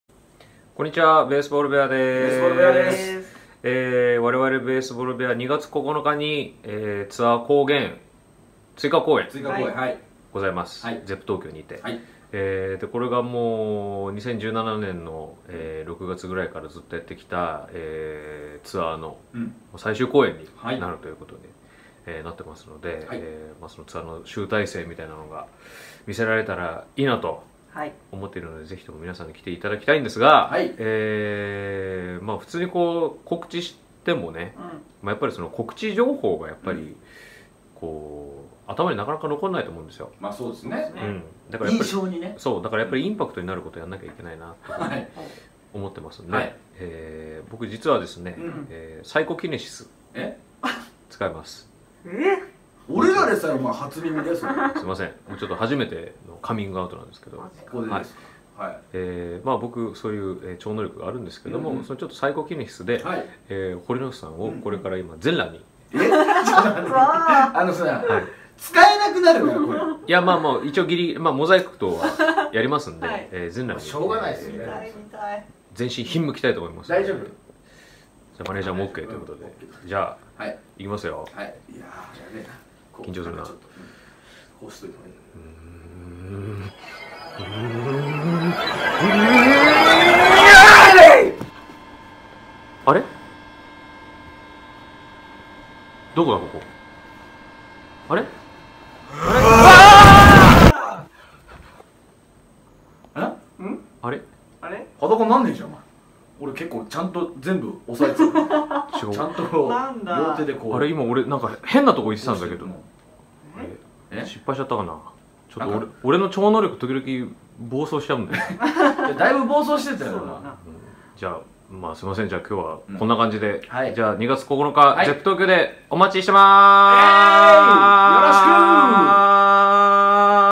【Base Ball Bear】2/9(金) Tour「光源」追加公演に向けて、スペシャルコメント動画到着！
■Tour「光源」追加公演
2018年2月9日(金) Zepp Tokyo